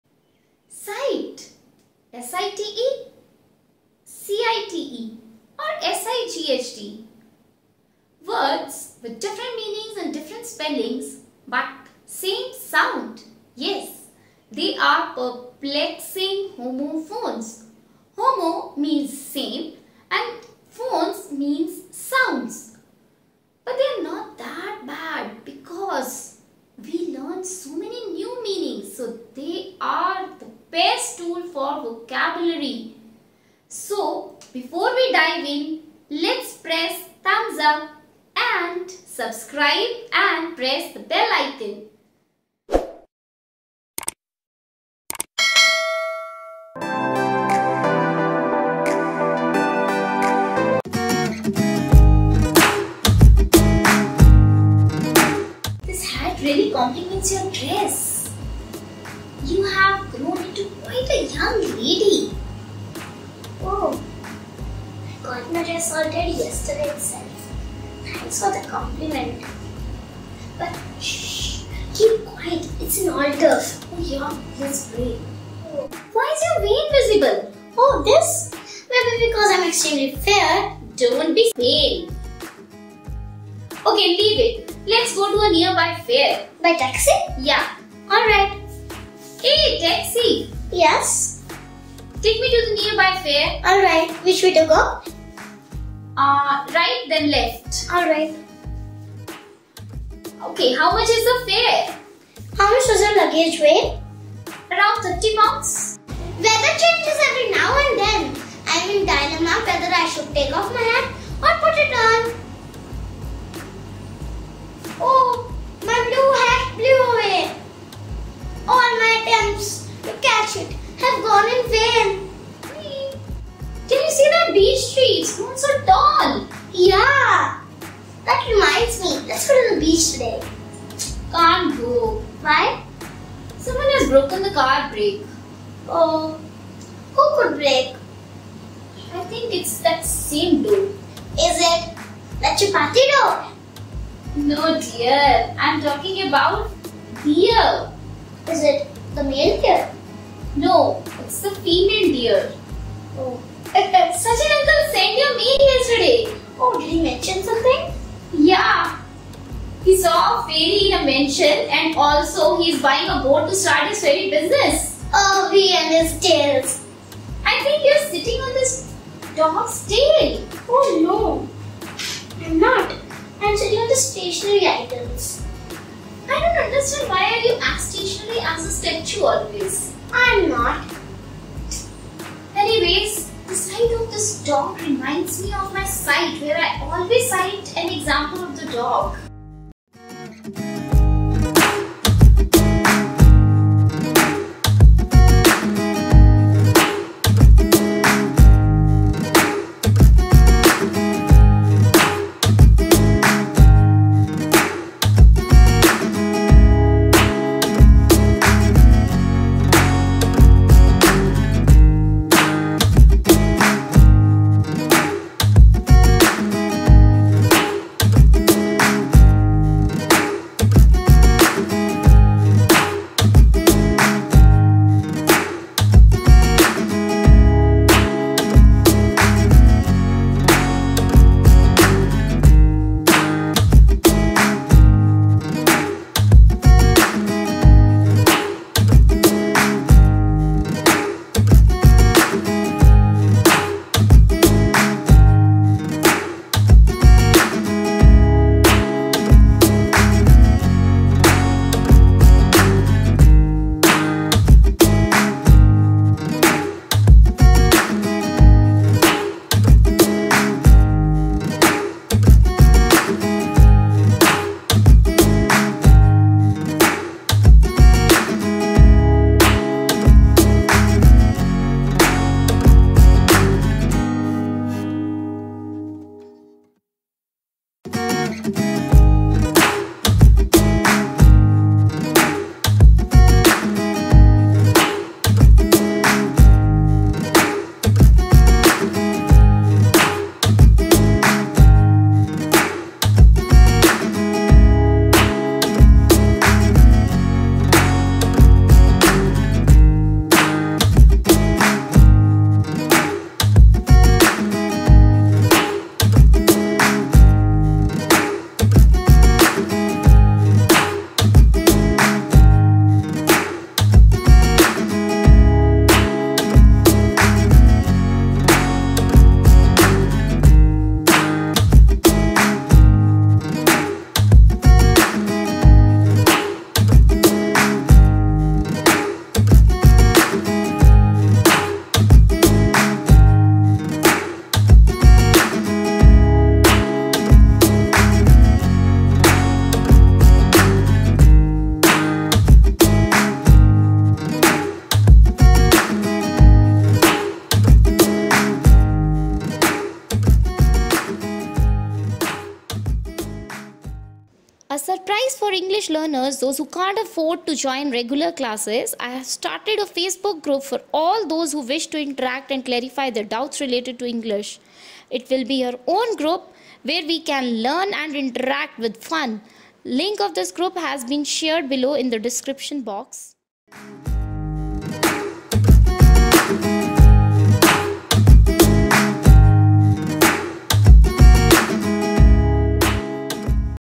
Homophones - Definition and Examples [ pronunciation and spellings of confusing words]
Homophones are the English words which have different spellings but same sounds and have the same pronunciation. This distinction can become really confusing, and can result in us using these words in the wrong way.
Do have a look at this video where we cover some most confusing but useful homophones both by simulating real life scenarios as well as illustrating them with the help of pictures.
So lets learn English the easier way.
please join this Facebook group as referred in the video to clarify your doubts and learn English :-)